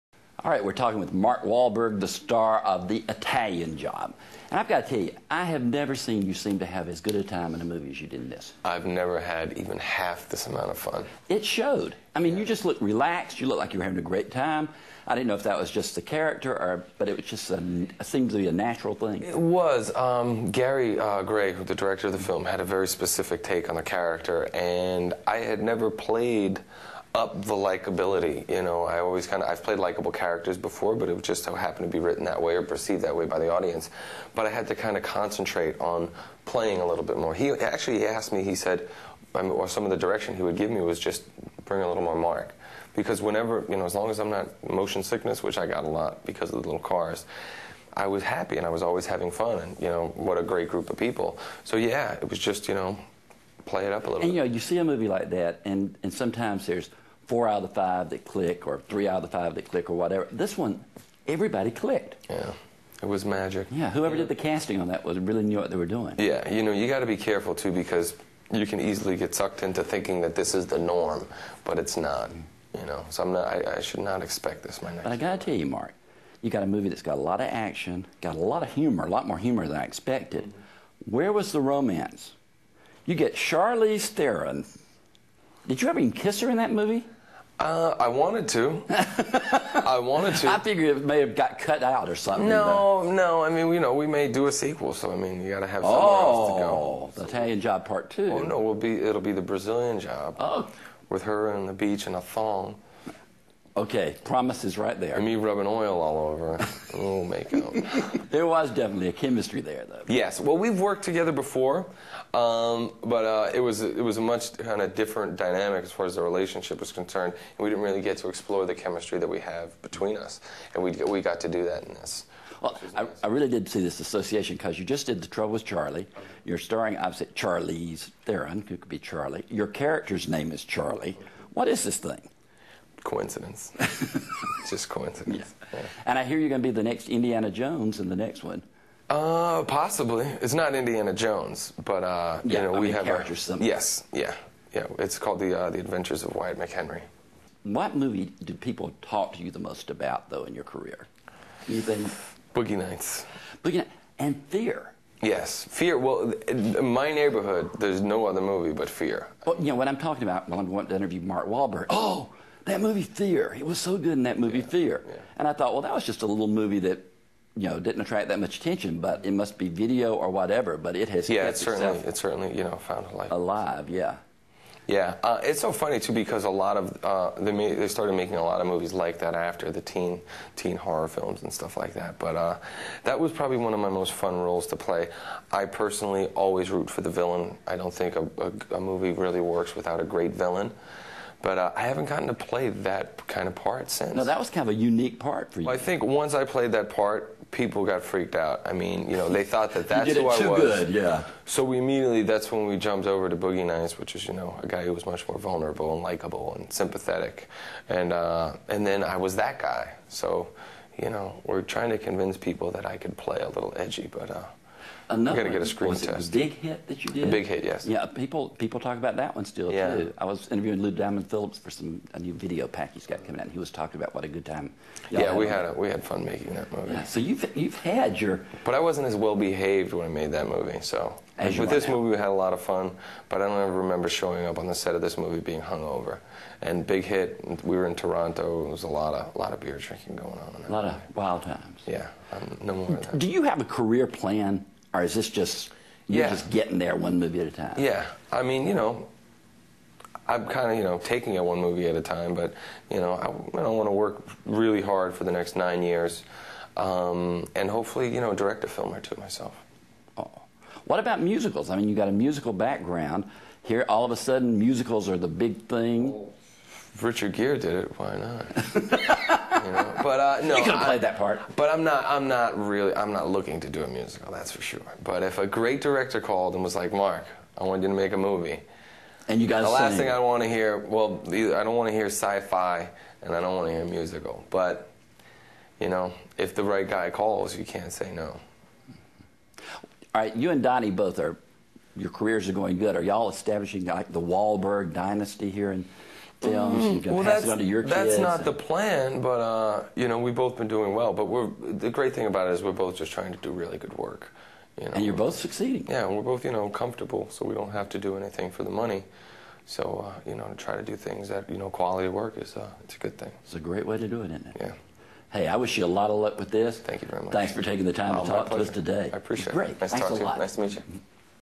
Archive-Mark Wahlberg Interview
Jackie K. Cooper interviews Mark Wahlberg, star of "The Italian Job."